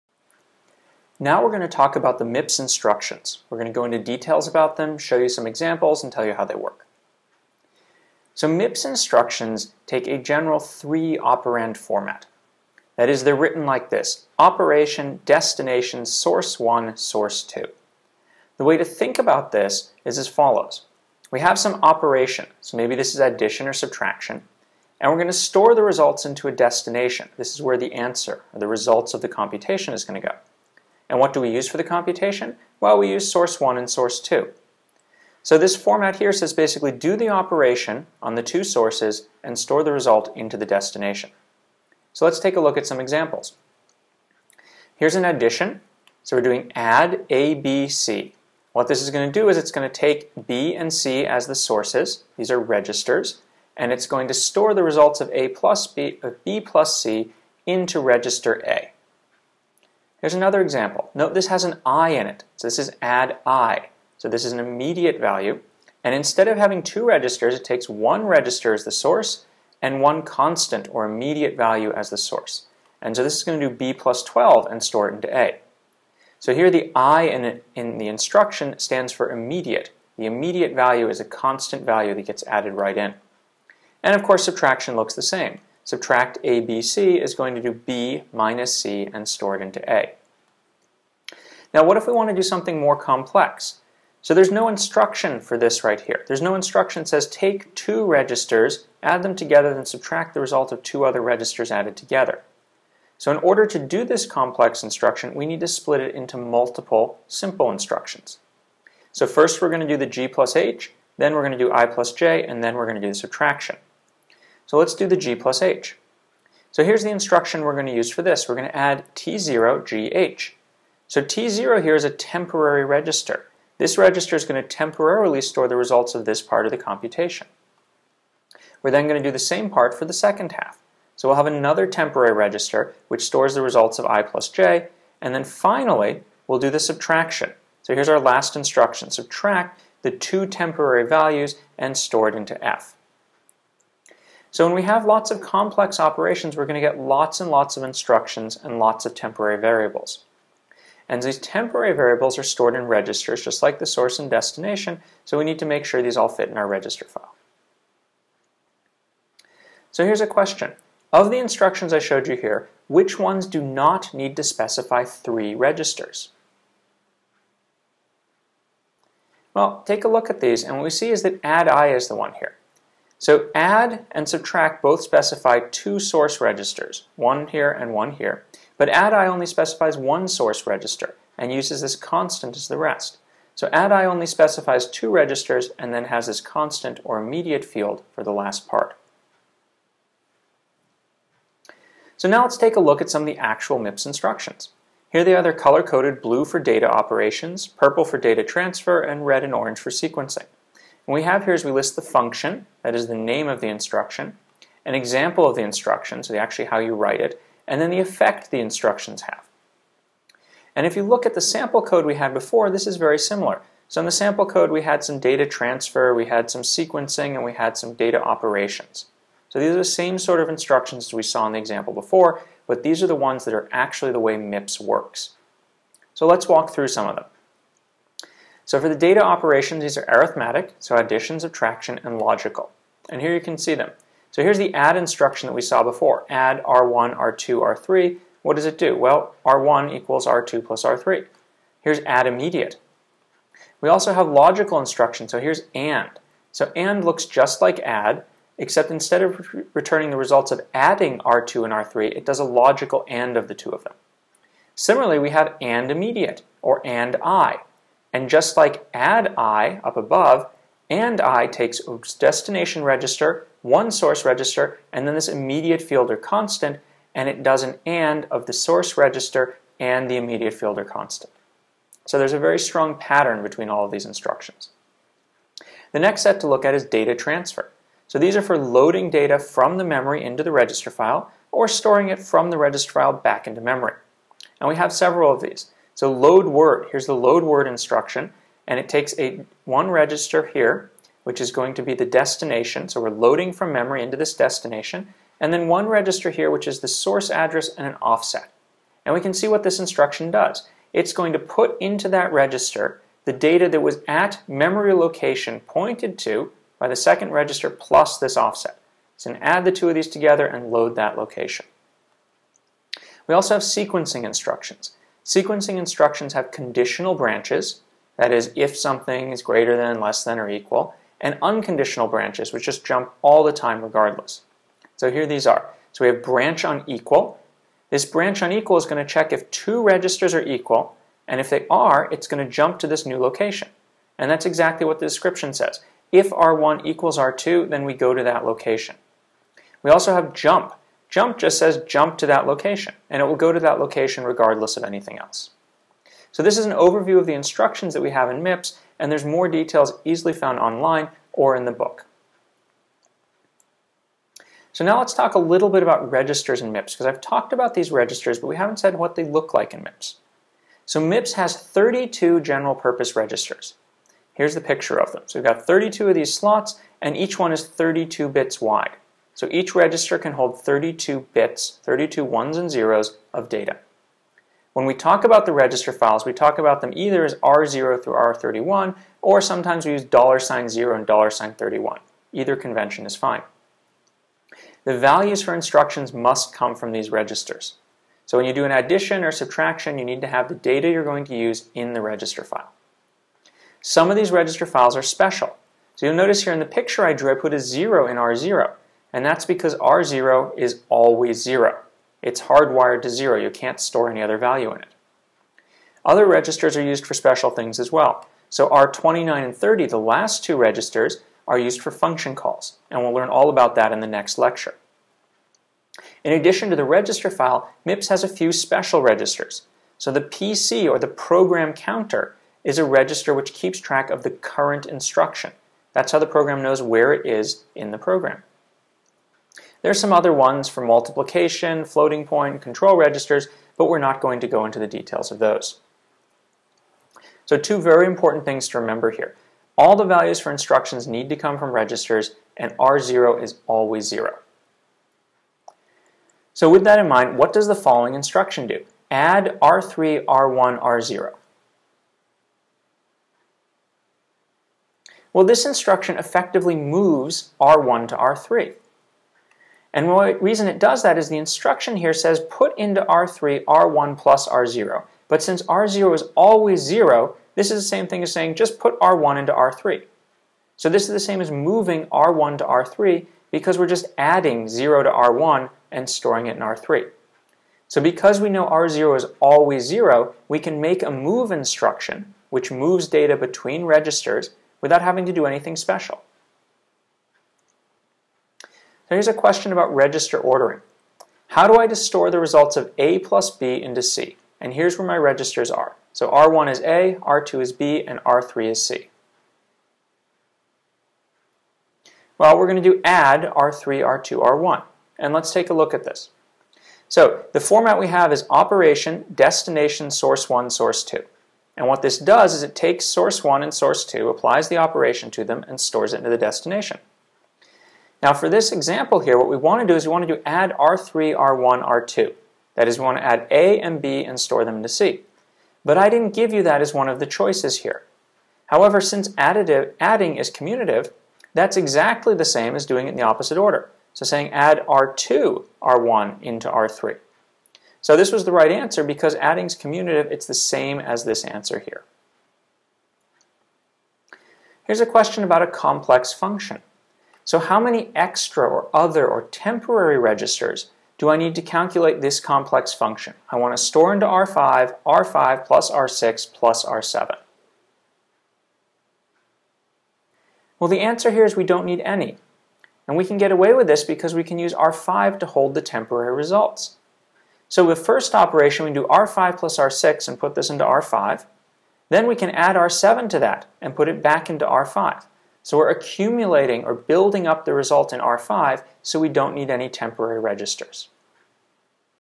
ISA 1.2 MIPS Instructions
enrollment key YRLRX-25436.
Contents: MIPS 3-operand format, add, addi, MIPS register file, R0.